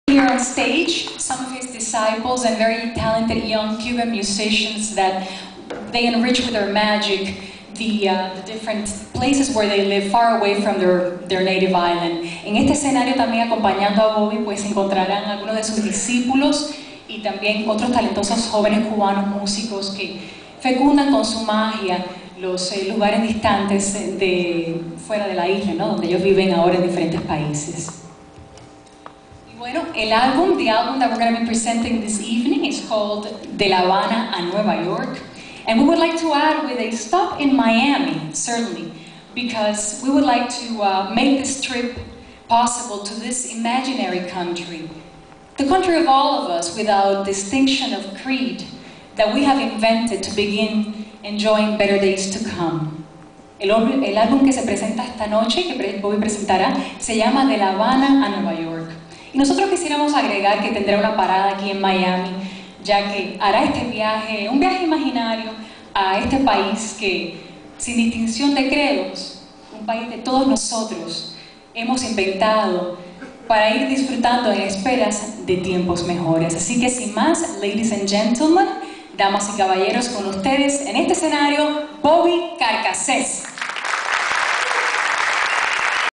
Viviam Maria Lopez - Bobby Carcasses Concert MC & Presentation - Part 2
MC & host Viviam Maria Lopez presents the debut concert of singer/composer/showman Bobby Carcasses in Teatro Manuel Artime, Miami, FL. (Part #2)
La locutora Viviam Maria Lopez presenta el concierto de Bobby Carcasses en el Teatro Manuel Artime de Miami, FL. (Parte 2)